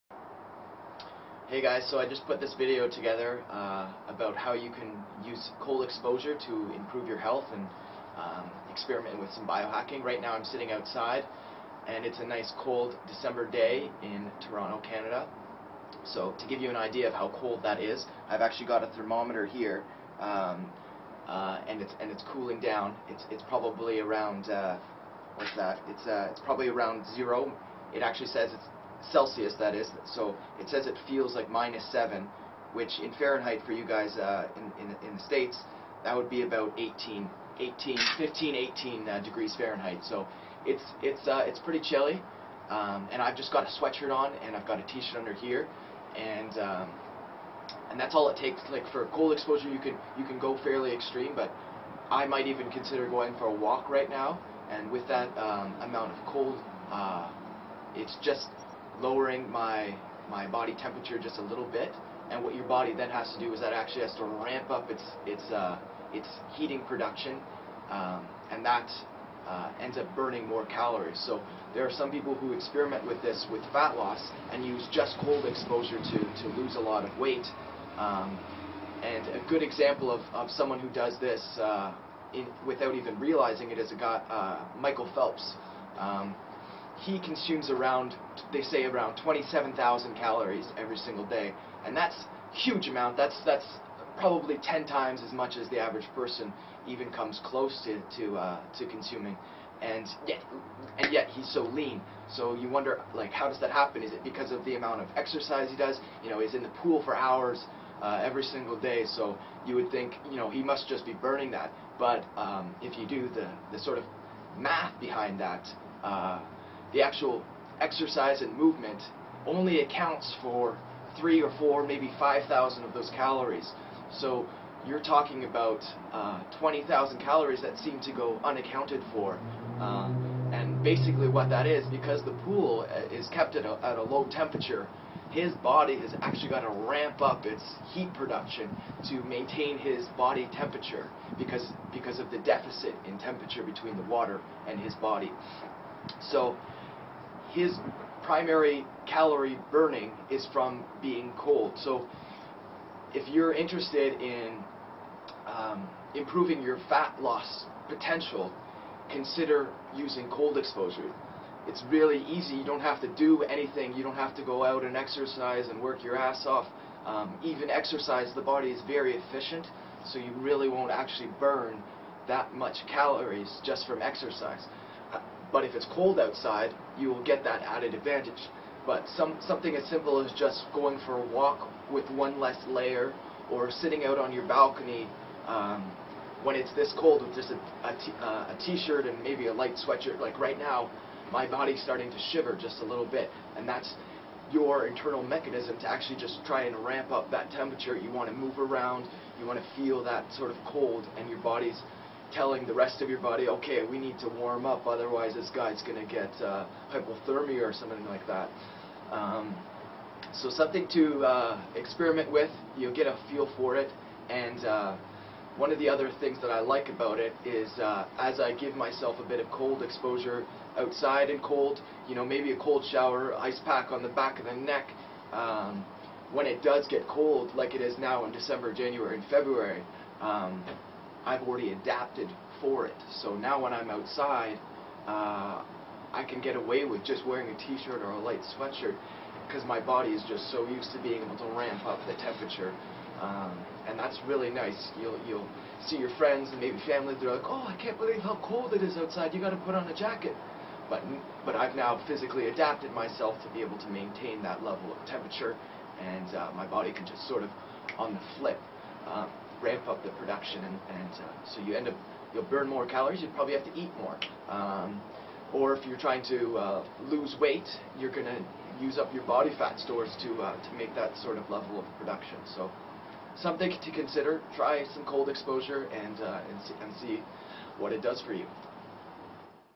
Biohacking with Cold Exposure - Easy Fat Loss
Outside in the cold explaining Cold exposure benefits and Fat loss.